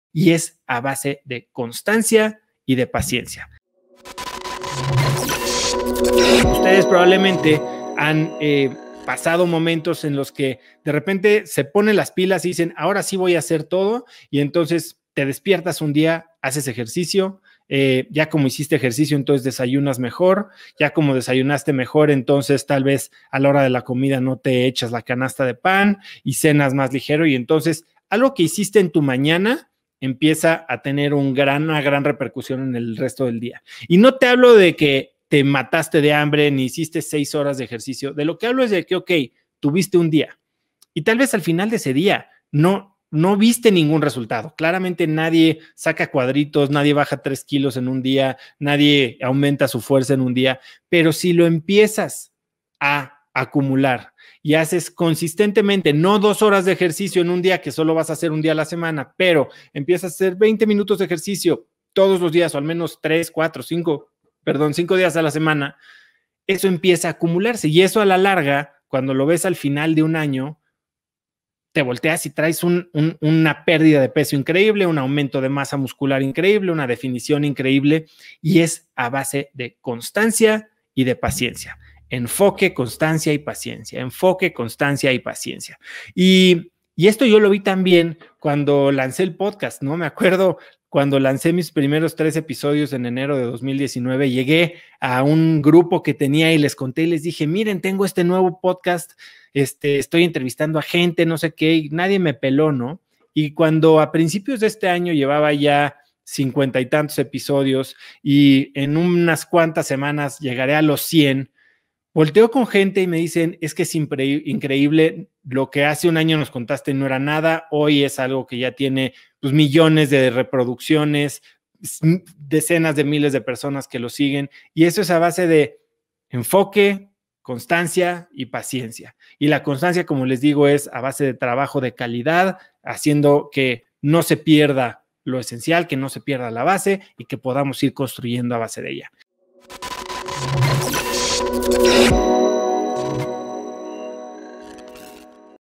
¡No te mates! Haz poquito
No tienes que matarte un solo día para lograr lo que quieres.
Se trata de hacer poco, pero acumularlo exponencialmente.
Enfoque + Constancia + Paciencia. (Repítelo 365 veces al año)
Verás los resultados exponenciales.